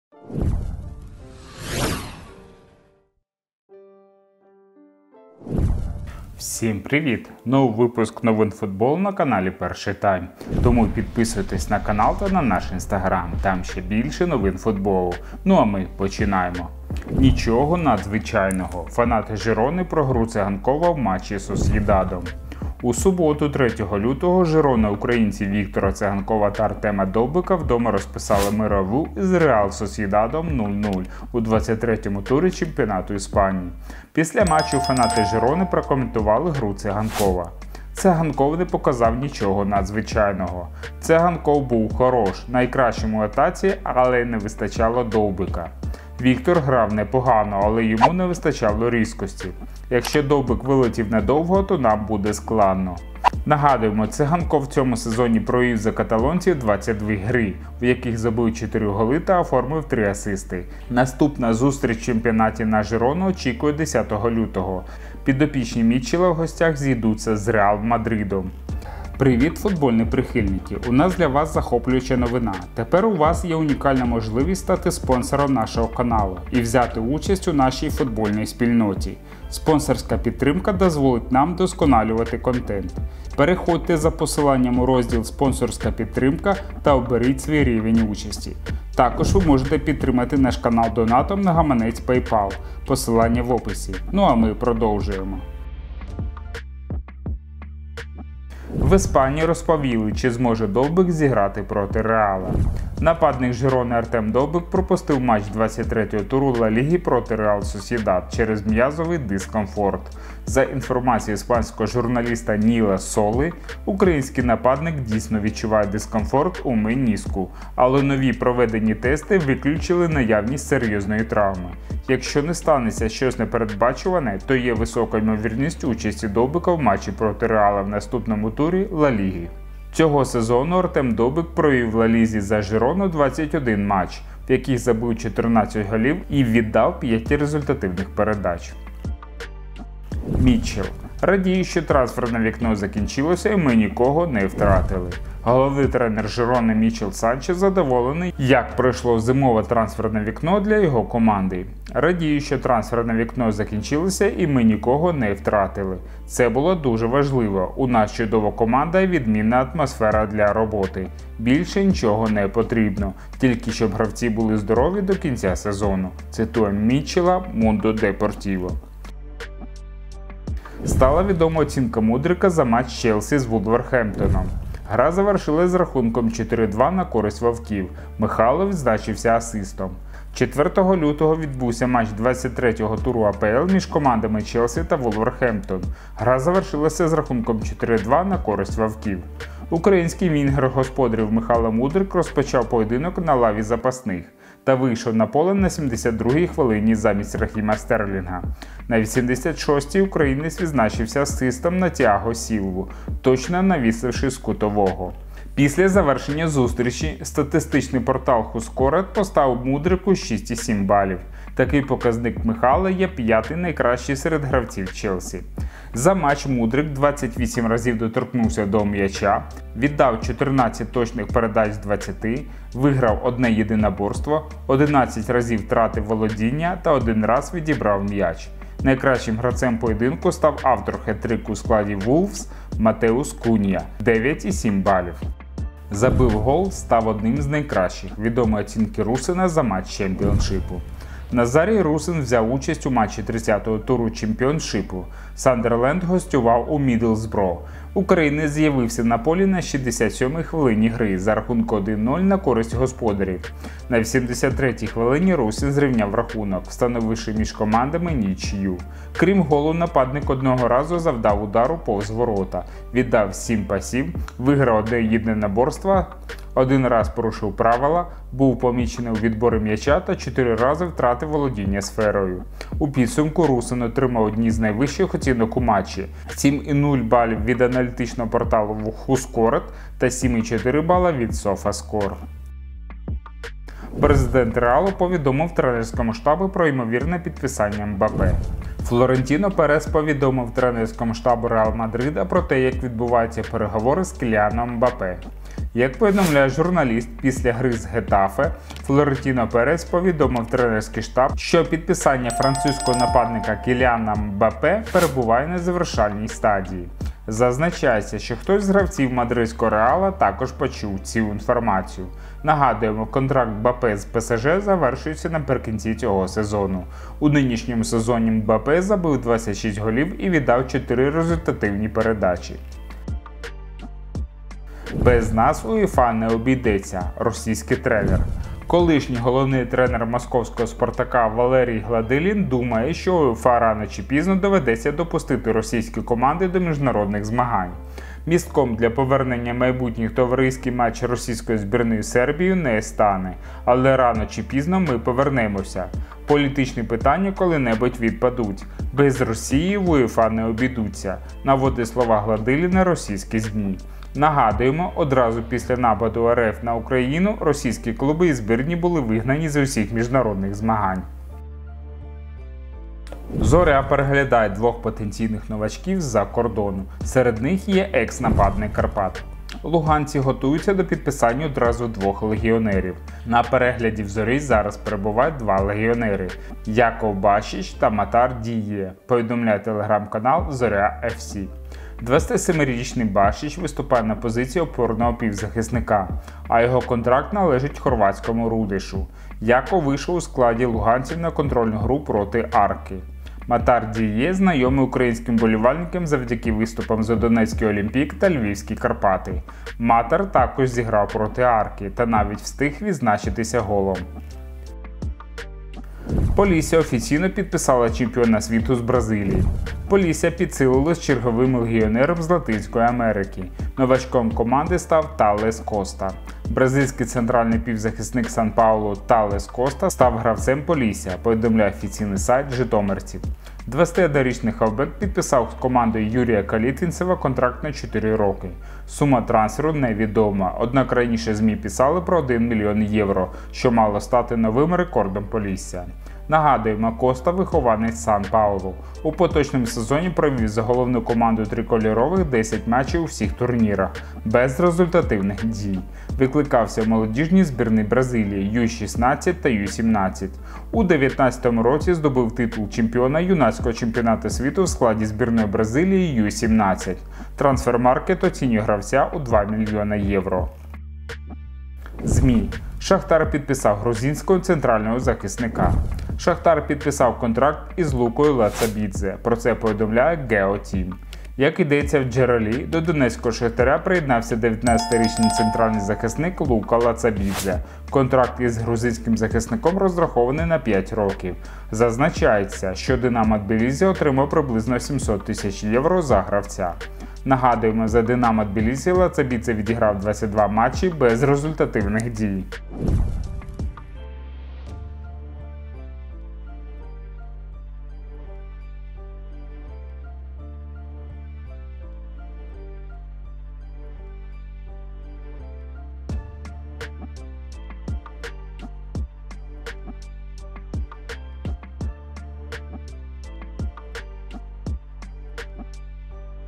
ТРАВМА ДОВБИКА ВИЯВИЛАСЯ НЕ СЕРЙОЗНОЮ! РУСИН ЗАБИВ ГОЛ І ВРЯТУВАВ СВОЮ КОМАНДУ. ВИПУСК № 149
Новини українського та світового футболу на каналі ПЕРШИЙ ТАЙМ.
Перший Тайм в Instagram - @pershij_taim
Перший Тайм в Тік Ток - @pershij_taim
Випуск 149

В цьому випуску:
1. Фанати Жирони про Циганкова
2. Травма Довбика не серйозна
3. Санчес про трансфери Жирони
4. Мудрик зробив асист за Челсі
5. Русин забив вирішальний гол за Сандерленд
6. Реал купує Мбаппе
7. Росіяни погрожують УЄФА
8. Трансфери Зорі
9. Полісся купує перспективного Бразилця